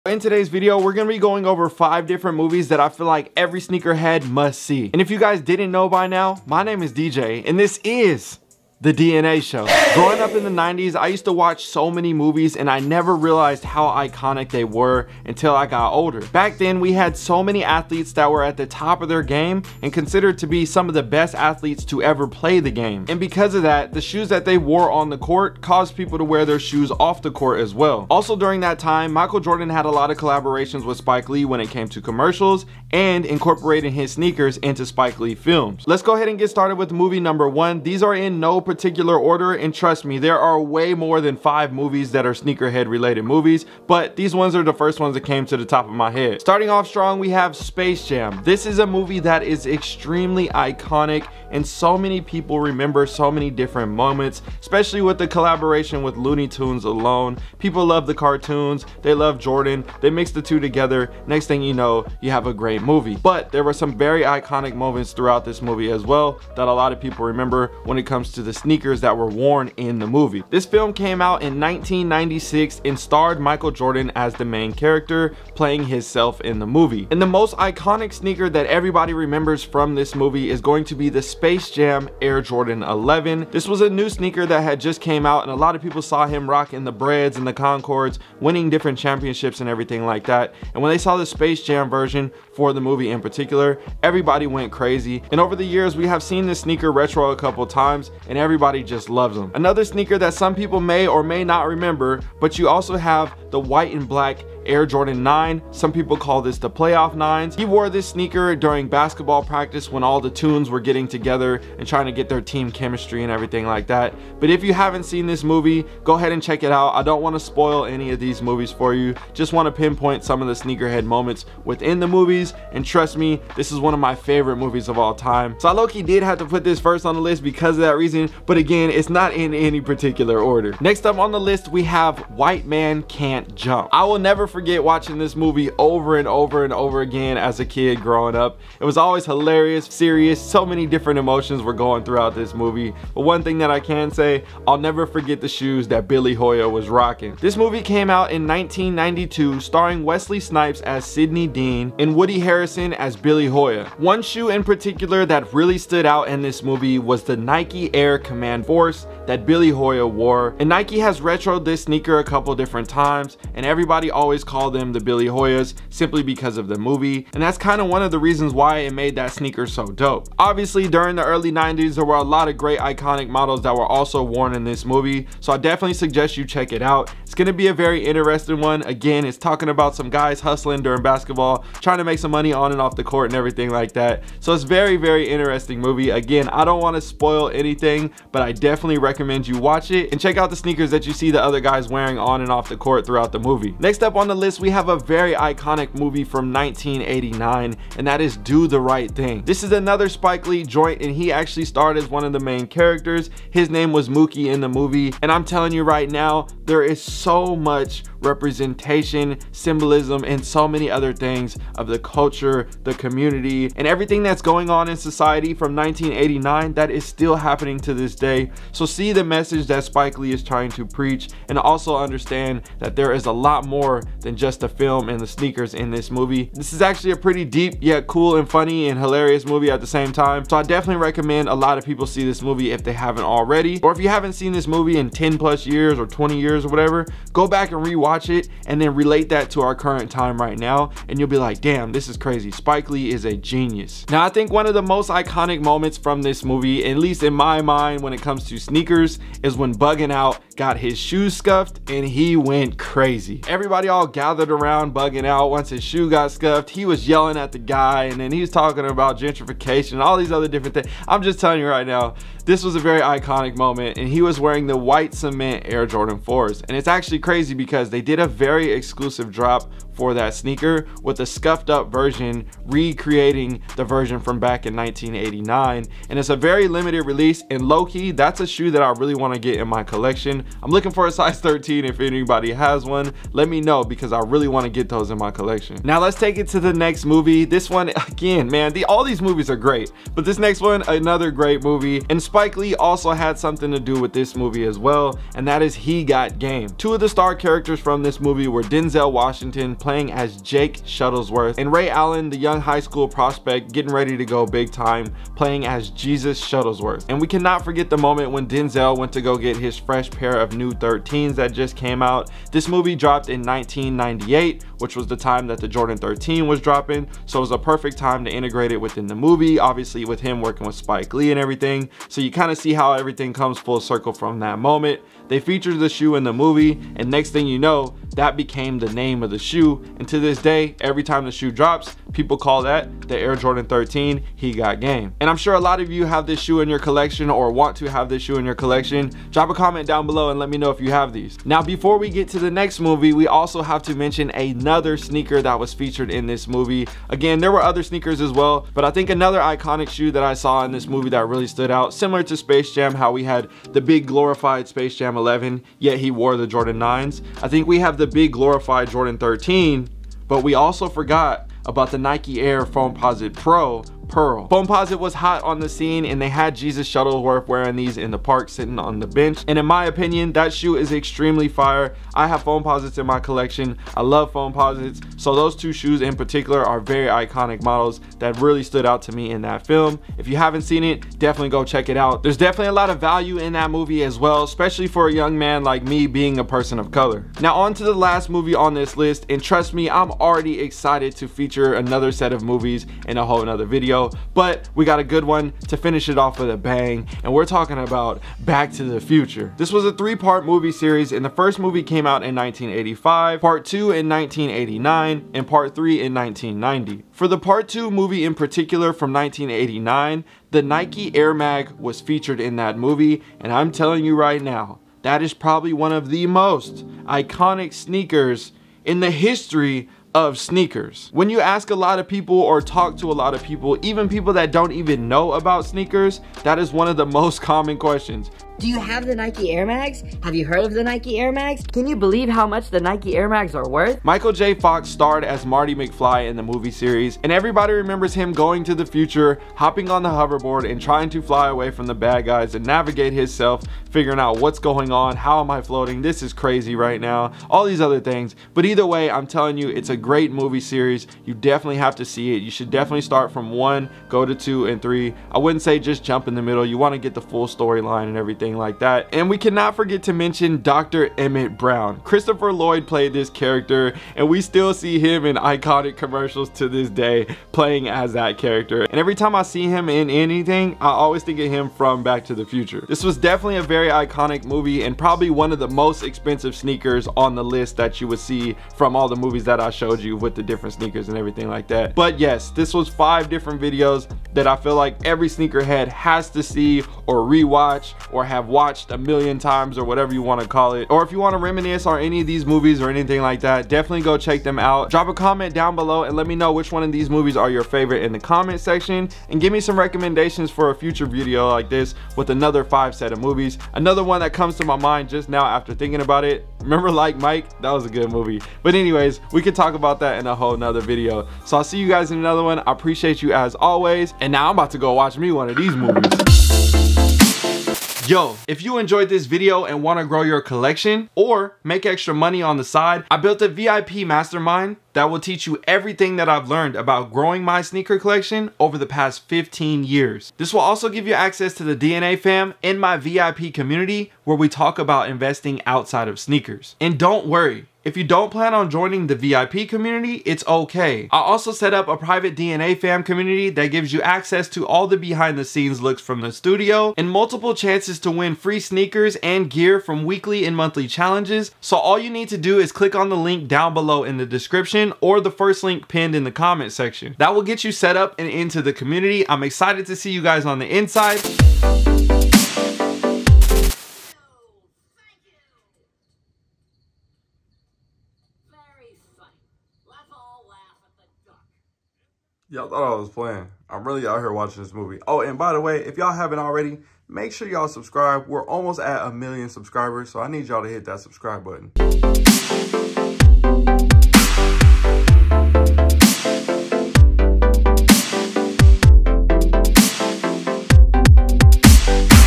5 Movies Every Sneakerhead MUST WATCH
5 Movies Every Sneakerhead MUST WATCH

THE LIST:
1) Space Jam
2) Do The Right Thing
3) White Man Can't Jump
4) He Got Game
5) Back To The Future

Start Your Own AIRBNB Business Today (Get $25 Free For Signing Up)